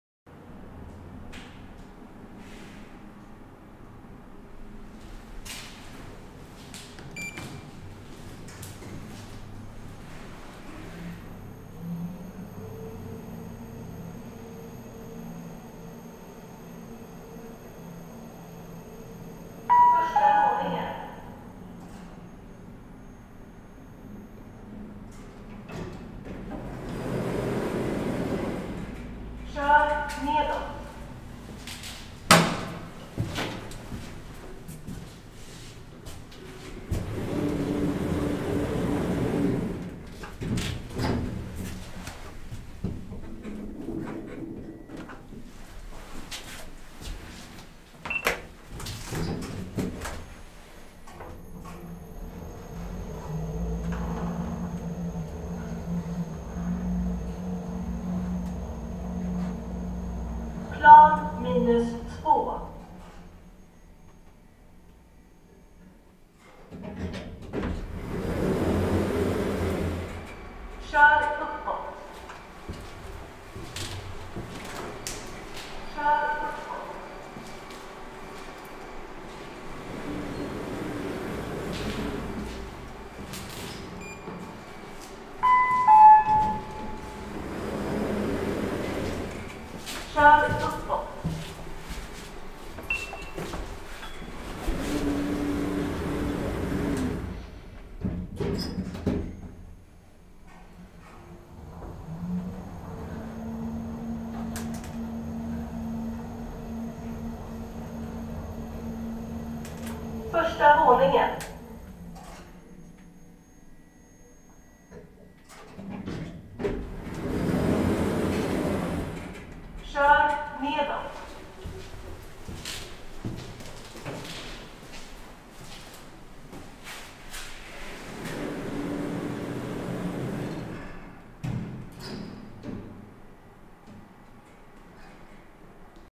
2017 KONE MonoSpace MRL-Traction Parking Garage Elevator @ Kvarnholmens Centrum
ELEVATOR INFORMATION!

Manufacturer/Tillverkare: KONE
Model/Modell: MonoSpace
Installation year/Byggår: 2017
Capacity/Maxvikt: 1000Kg - 13 Persons
Floors/Antal våningar: 4 = -2 -1 E 1

Location/Adress: Kvarnholmens Centrum Parking Garage | Kvarnholmsvägen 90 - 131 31 Nacka (Kvarnholmen) SWEDEN!